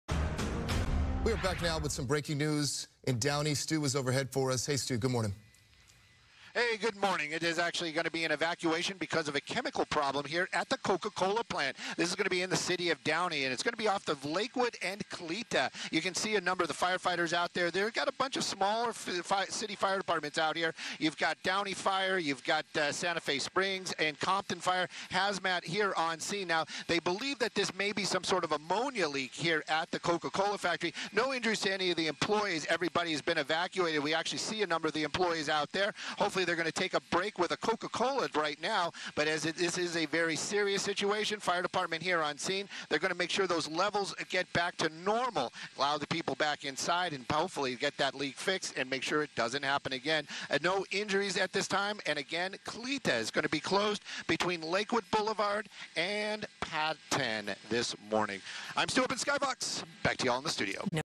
Evacuations at Coca Cola facility
The plant was evacuated over a possible ammonia leak.